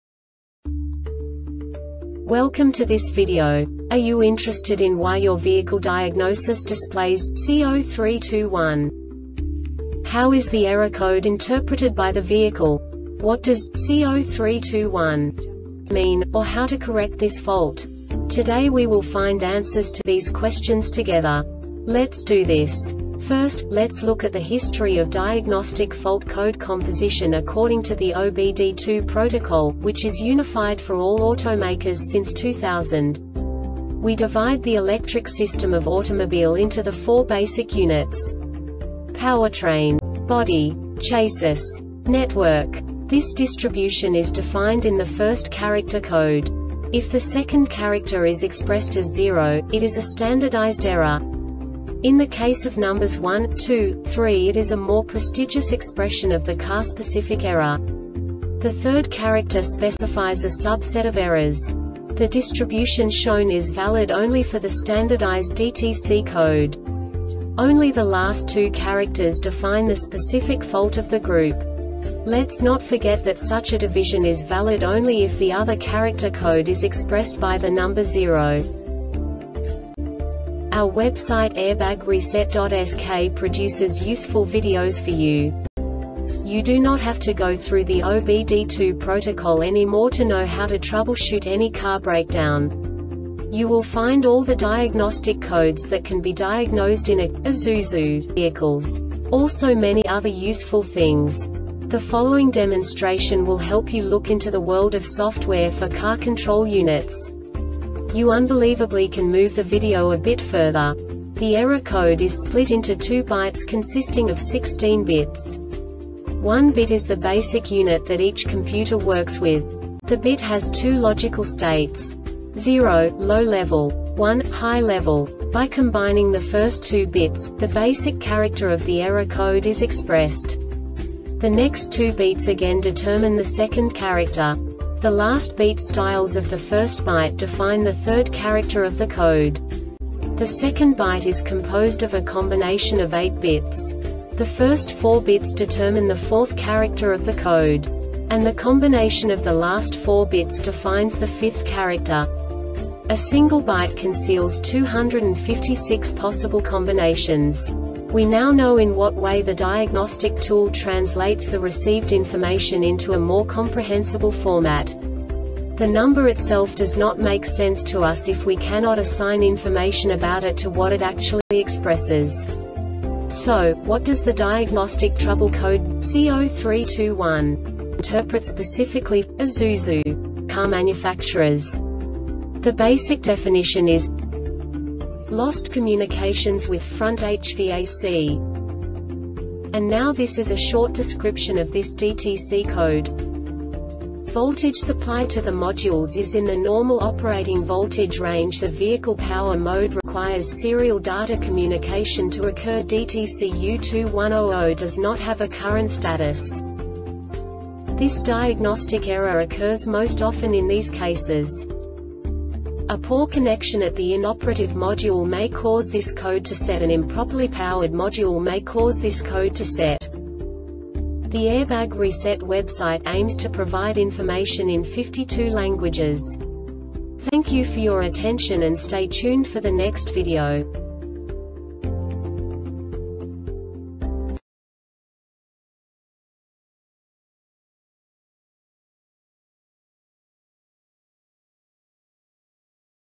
DTC Isuzu C0321 Short Explanation
Contents:
0:21 Basic DTC analysis according to OBD2 protocol standard.
1:48 Insight into programming
2:58 A diagnostically understandable description of the Diagnostic Trouble Code
3:05 Basic error definition

Make:
Isuzu
Code:
C0321
Definition:
Lost Communications With Front HVAC
Description:
Voltage supplied to the modules is in the normal operating voltage range.The vehicle power mode requires serial data communication to occur.DTC U2100 does not have a current status.